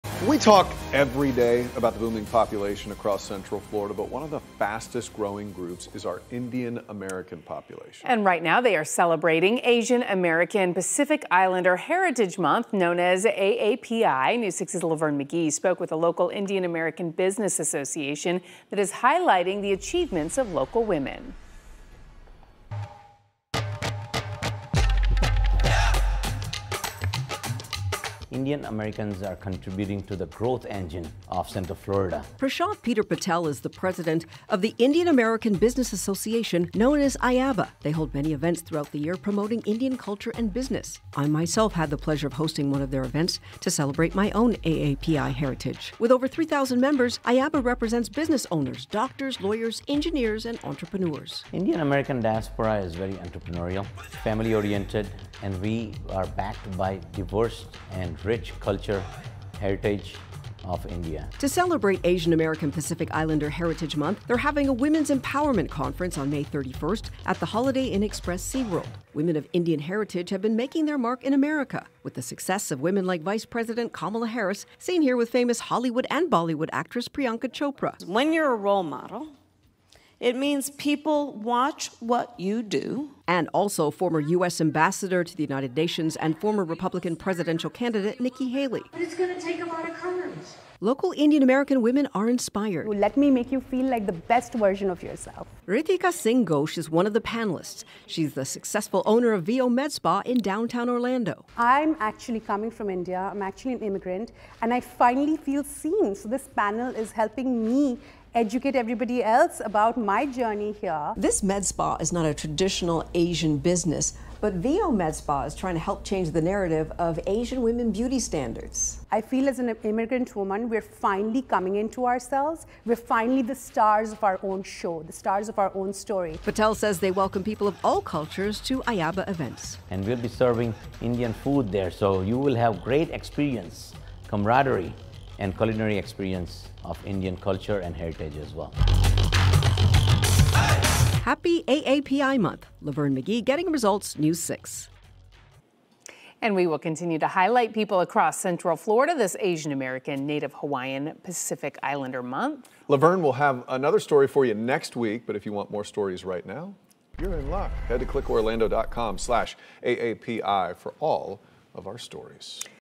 Indian Americans a fast growing community in Central Florida
The Indian American Business Association has over 3,000 members, and is celebrating Indian culture in Central Florida this month.